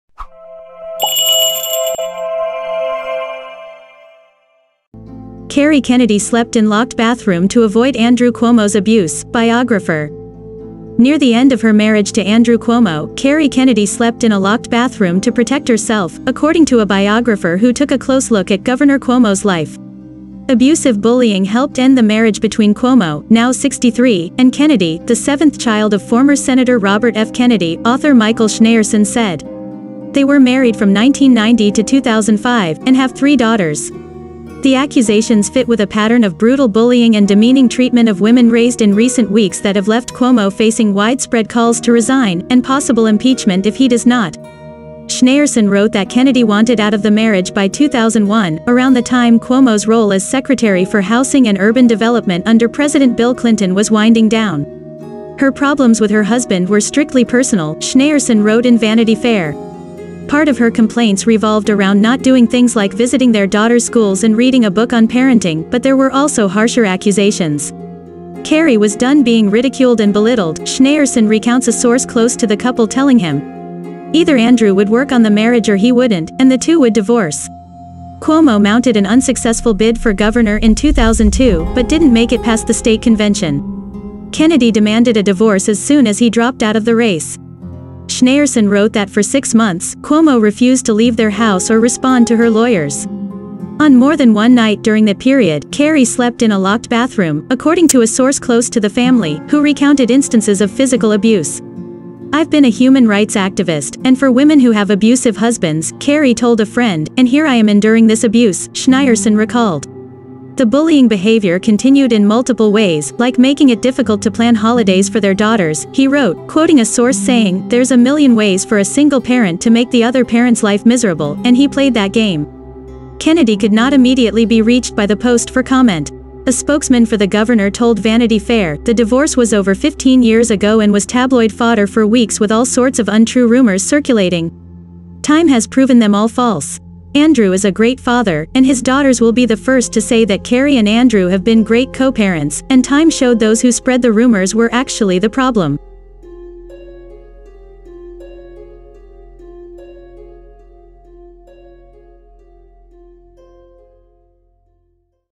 Kerry Kennedy slept in locked bathroom to avoid Andrew Cuomo
Near the end of her marriage to Andrew Cuomo, Kerry Kennedy slept in a locked bathroom to protect herself, according to a biographer who took a close look at Gov. Cuomo’s life.